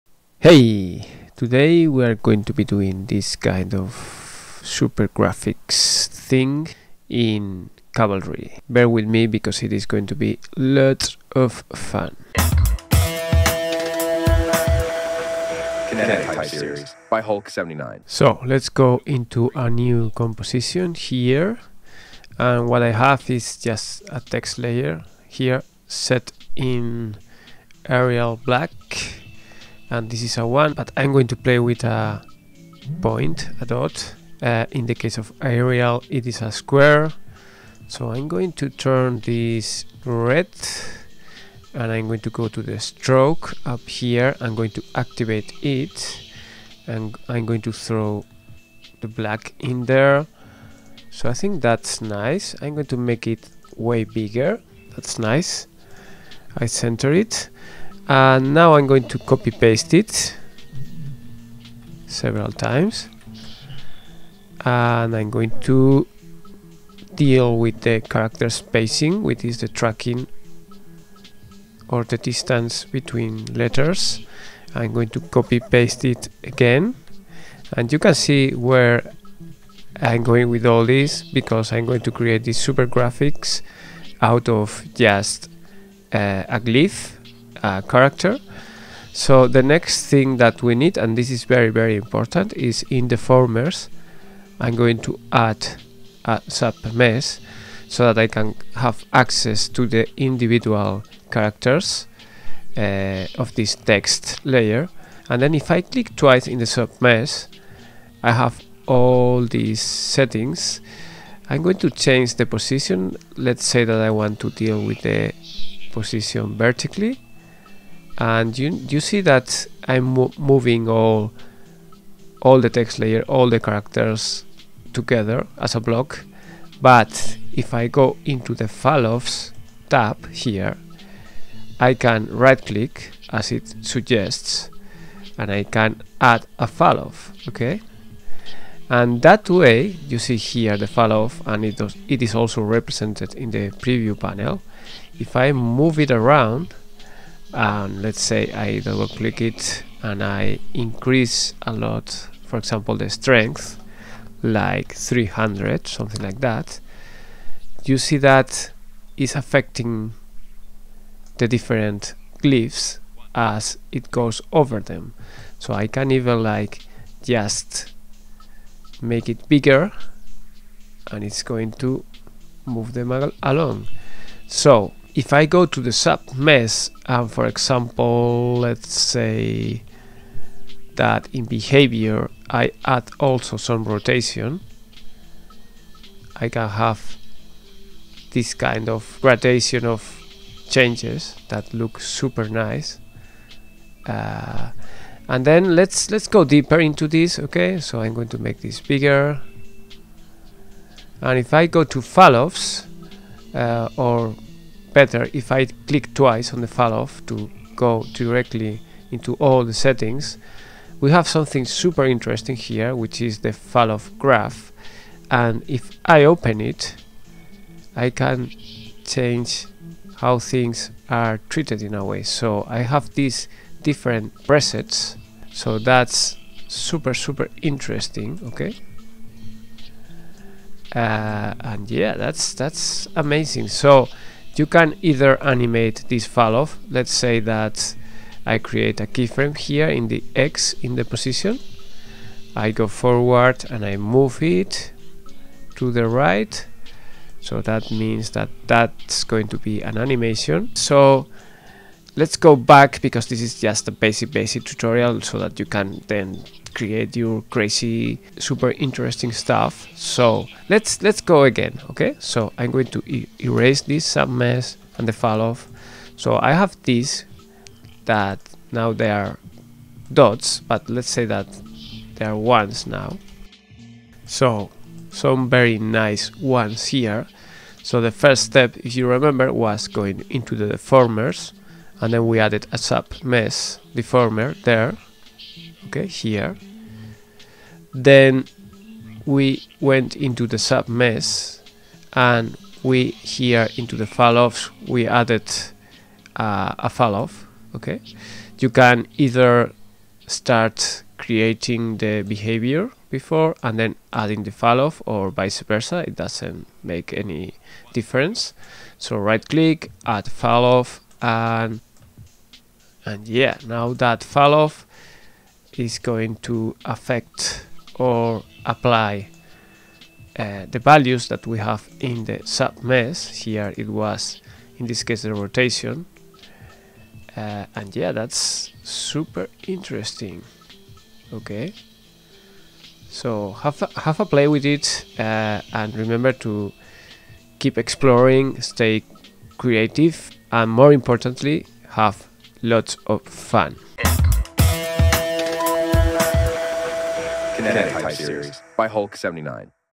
KTS® - Super Graphics - Cavalry
I do the channel for free and in my spare time. If you want to consider paying something, I will appreciate your contribution a lot:
In this tutorial we will play around with Cavalry.
Dealing with Sub-Mesh to have control over the glyphs on a text and then creating a Falloff to create a gradation of changes. The result: Super Graphics!

  / holke79